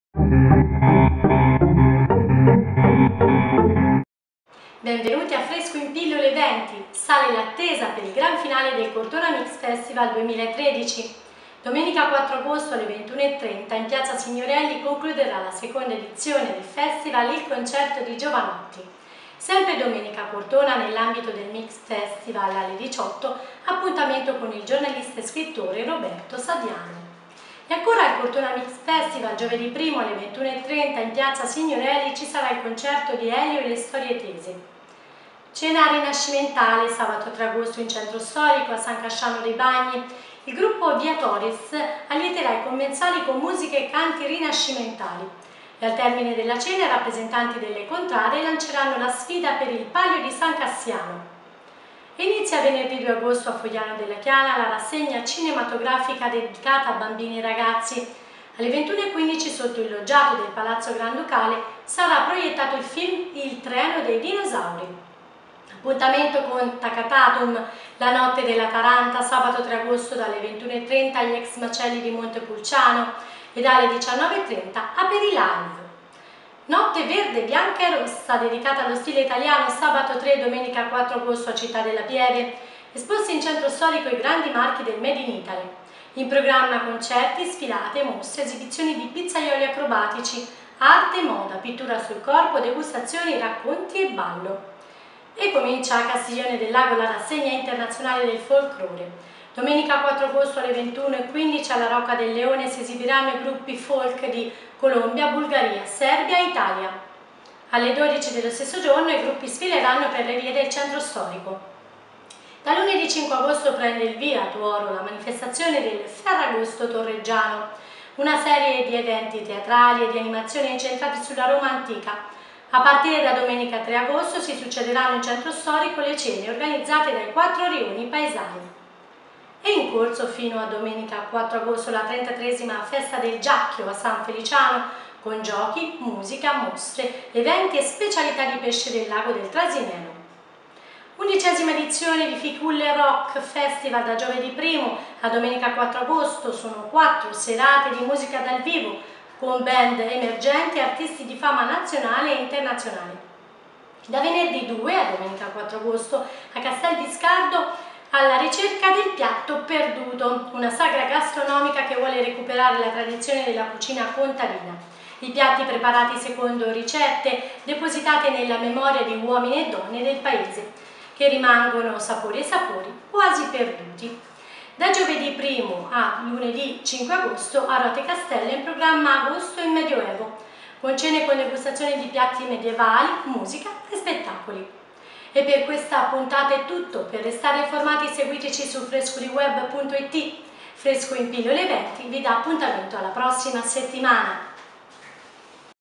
Fresco in Pillole EVENTI-Settimana dal 1° all'8 agosto
Fresco in Pillole EVENTI, il tg di aggiornamento settimanale degli eventi di Trasimeno, Valdichiana e Orvietano. Gli appuntamenti della settimana dall'1 all'8 agosto 2013. Conduce Francesca Bennati